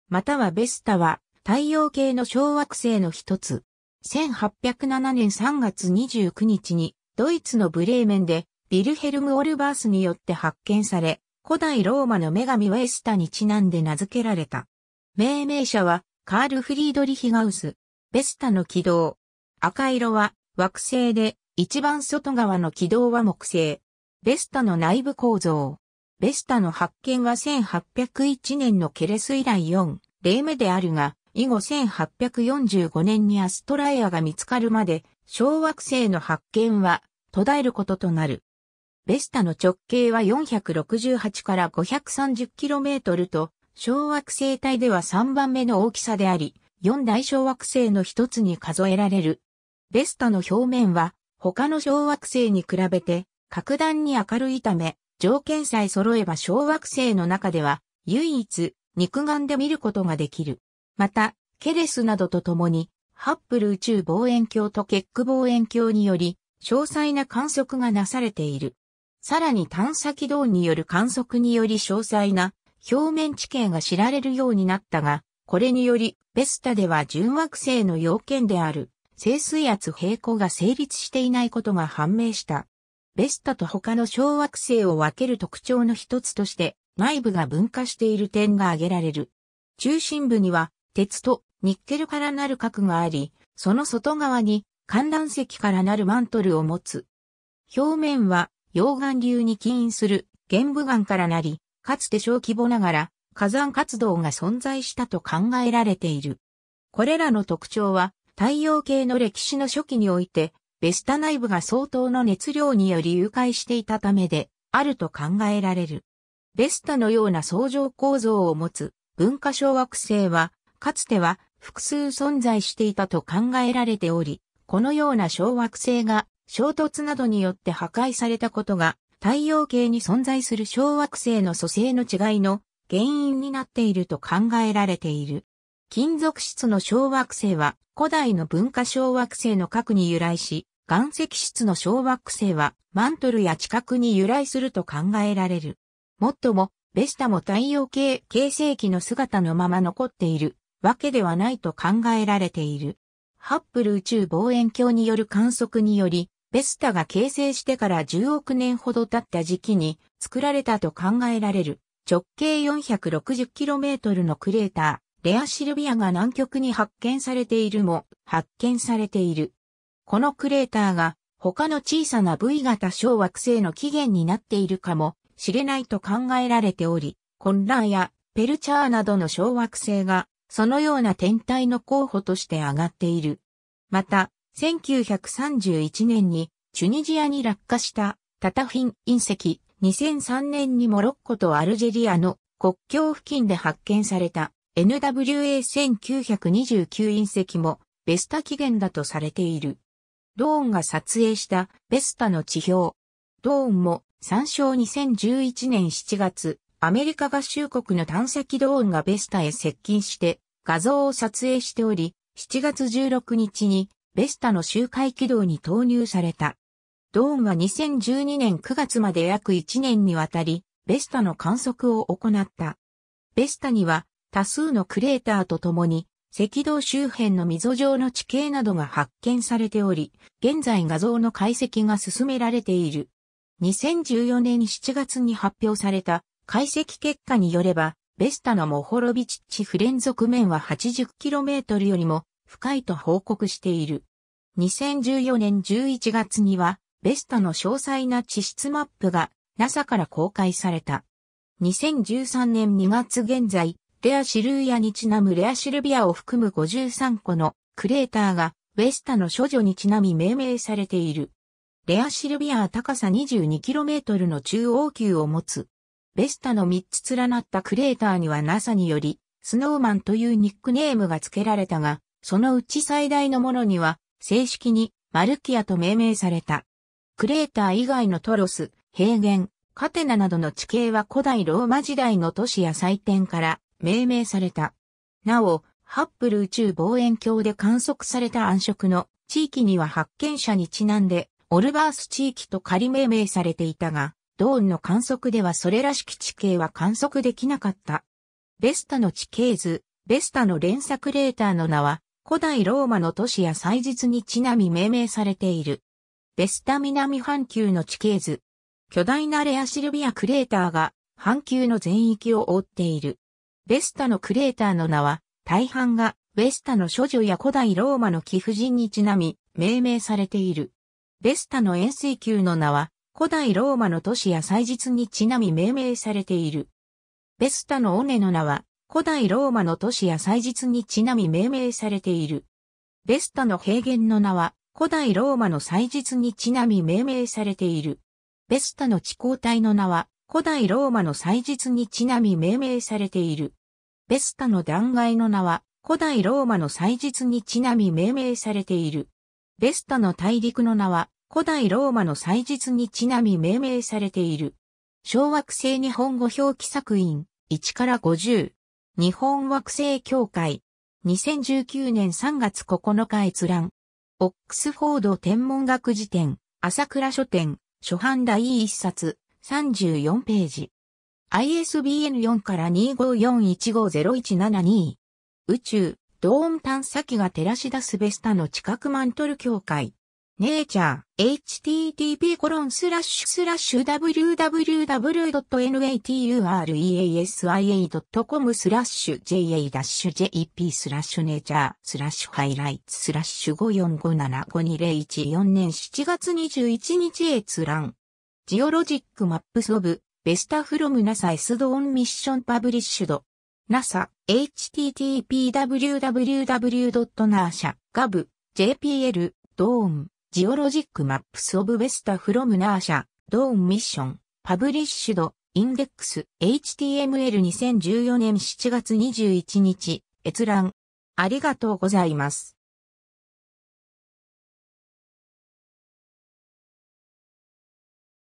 ベスタ (小惑星)
ベスタ またはヴェスタ (4 Vesta) は、太陽系の小惑星の一つ。
1807年3月29日にドイツのブレーメンでヴィルヘルム・オルバースによって発見され、古代ローマの女神ウェスタにちなんで名付けられた。
命名者はカール・フリードリヒ・ガウス。
 ベスタ（青色）の軌道。
赤色は惑星で、一番外側の軌道は木星。
 ベスタの内部構造。
 ベスタの発見は1801年のケレス（2006年以降は準惑星）以来4例目であるが、以後1845年にアストラエアが見つかるまで小惑星の発見は途絶えることとなる。
ベスタの直径は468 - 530kmと、小惑星帯（メインベルト）では3番目の大きさであり、四大小惑星の一つに数えられる。
ベスタの表面は他の小惑星に比べて格段に明るいため、条件さえそろえば小惑星の中では唯一、肉眼で見ることができる。
また、ケレスなどとともにハッブル宇宙望遠鏡とケック望遠鏡により詳細な観測がなされている。
さらに探査機ドーンによる観測（後述）により詳細な表面地形が知られるようになったが、これによりベスタでは準惑星の要件である静水圧平衡が成立していないことが判明した。
 ベスタと他の小惑星を分ける特徴の一つとして、内部が分化している（地球のような層状構造を持つ）点が挙げられる。
中心部には鉄とニッケルからなる核があり、その外側にカンラン石からなるマントルを持つ。
表面は溶岩流に起因する玄武岩からなり、かつて小規模ながら火山活動が存在したと考えられている。
これらの特徴は、太陽系の歴史の初期においてベスタ内部が相当の熱量により融解していたためであると考えられる。
 ベスタのような層状構造を持つ分化小惑星は、かつては複数存在していたと考えられており、このような小惑星が衝突などによって破壊されたことが、太陽系に存在する小惑星の組成の違いの原因になっていると考えられている。
金属質の小惑星は、古代の分化小惑星の核に由来し、岩石質の小惑星はマントルや地殻に由来すると考えられる。
 もっとも、ベスタも太陽系形成期の姿のまま残っているわけではないと考えられている。
ハッブル宇宙望遠鏡による観測により、ベスタが形成してから10億年ほど経った時期に作られたと考えられる、直径460kmのクレーター、レアシルヴィア（Rheasilvia）が南極に発見されている（さらに、ドーンによりレアシルヴィアに隣接してさらに古いクレーター、ヴェネネイア（Veneneia、直径395km）も発見されている）。
このクレーターが他の小さなV型小惑星の起源になっているかもしれないと考えられており、(1929) コッラー (Kollaa) や (3850) ペルチャー (Peltier) などの小惑星がそのような天体の候補として挙がっている。
また、1931年にチュニジアに落下したタタフィン隕石 (Tatahouine meteorite)、2003年にモロッコとアルジェリアの国境付近で発見されたNWA 1929隕石もベスタ起源だとされている。
 ドーンが撮影したベスタの地表。
 「ドーン (探査機)」も参照 2011年7月、アメリカ合衆国の探査機ドーンがベスタへ接近して画像を撮影しており、7月16日にベスタの周回軌道に投入された。
ドーンは2012年9月まで約一年にわたりベスタの観測を行った。
 ベスタには多数のクレーターと共に、赤道周辺の溝状の地形などが発見されており、現在画像の解析が進められている。
 2014年7月に発表された解析結果によれば、ベスタのモホロビチッチ不連続面は80kmよりも深いと報告している。
（これまでは深さ30km程度と考えられていた） 2014年11月には、ベスタの詳細な地質マップがNASAから公開された。
 2013年2月現在、レア・シルウィアにちなむレアシルヴィア（すでに命名されている小惑星 (87) シルヴィアと区別するために結合されている）を含む53個のクレーターがウェスタの処女にちなみ命名されている。
レアシルヴィアは高さ22kmの中央丘を持つ。
ベスタの3つ連なったクレーターにはNASAにより"snowman"（雪だるま）というニックネームが付けられたが、そのうち最大のものには正式に「マルキア (Marcia)」と命名された。
 クレーター以外のトロス（ドーム状の山）、平原、カテナ（鎖状に連なったクレーター）などの地形は古...